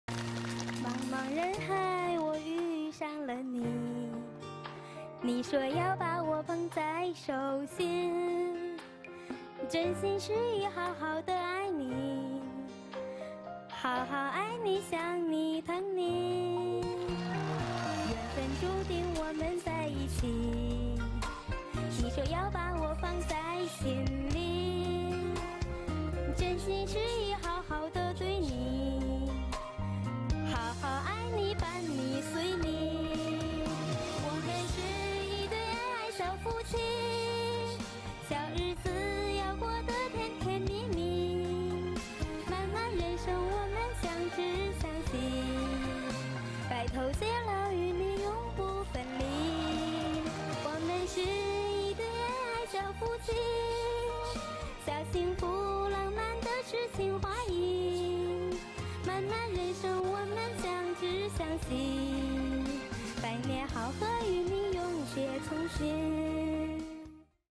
두 아이를 낳은 유리어머니|CCTV 한국어방송
허팅（何婷）은 선천성적인 불완전 골형성증이 있다 . 불완전 골형성증은 이유없이 쉽게 골절되는 희귀병이다. 그의 남편 임유롱도 차사고로 반신불수가 되었다. 어려운 생활에도 불구하고 부부사이는 서로를 의지하면서 행복하게 살아왔다. 불완전 골형성증의 유전 확율은 50%로 높지만 허팅은 기어이 딸과 아들을 낳았다. 다행인것은 두 자식이 건강하게 자라났다. CCTV의 무대를 빌어 허팅은 자기의 남편과 아들 딸에게 사랑의 노래를 선사해 관중들을 감동시켰다.